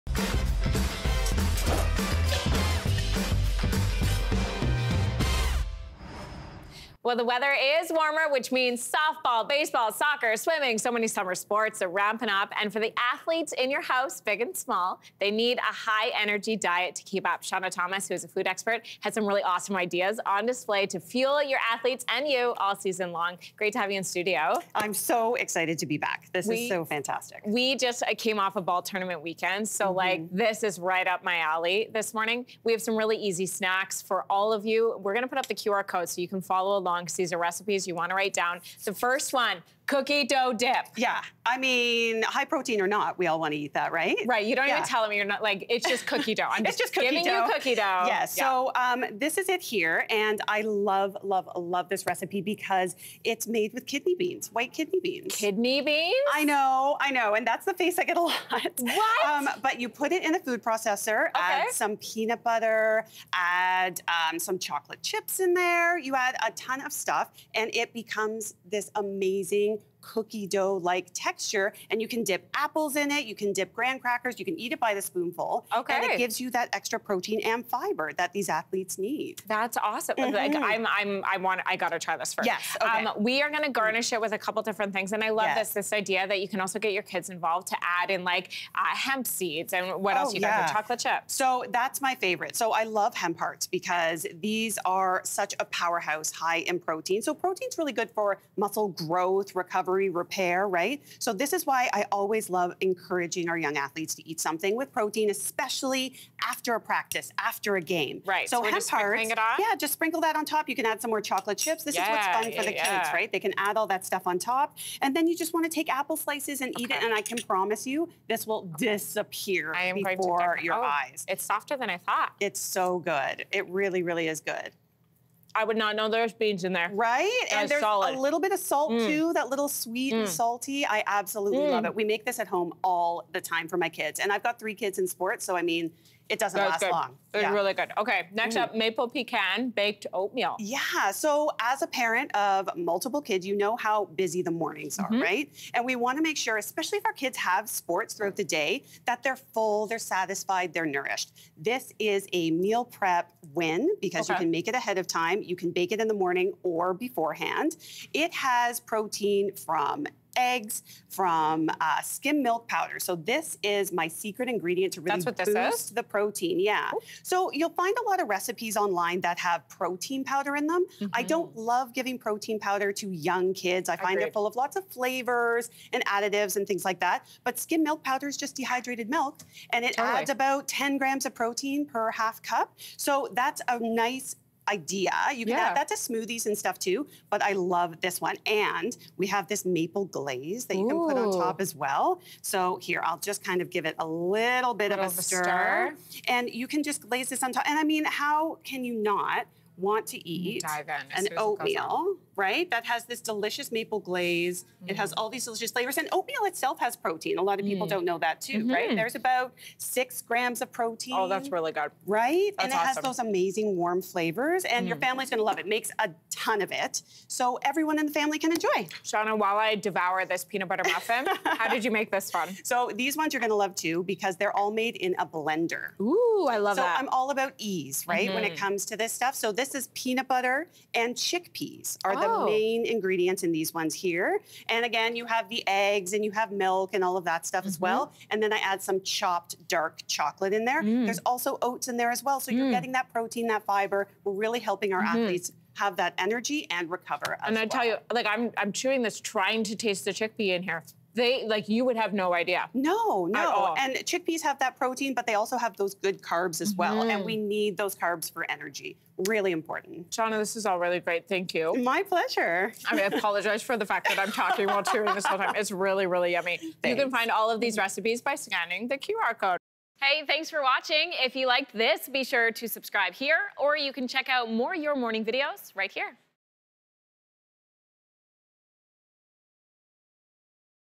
High Protein Snacks for Athletes | Your Morning
We take a look at some delicious healthy treats for the athlete in your house!